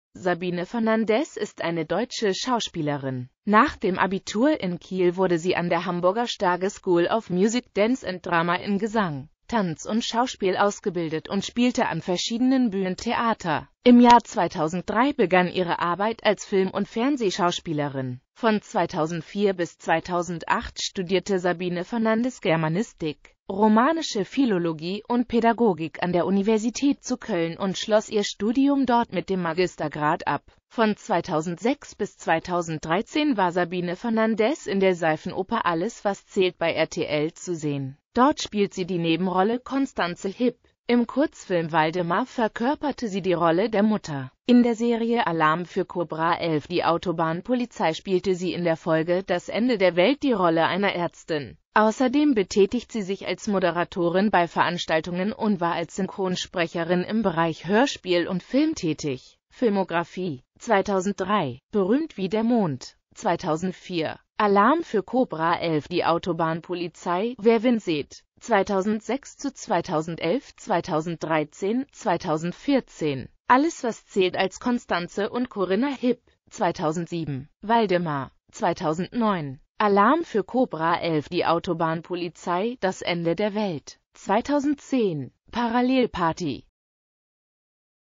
Sabine Fernandez
Vielen Dank für Ihre Unterstützung:

Sabine Fernandez (* 19.August 1978 in Hannover) ist eine deutsche Schauspielerin.Nach dem Abitur in Kiel wurde sie an der Hamburger Stage School of Music, Dance and Drama in Gesang, Tanz und Schauspiel ausgebildet und spielte an verschiedenen Bühnen Theater.Im Jahr 2003 begann ihre Arbeit als Film- und Fernsehschauspielerin.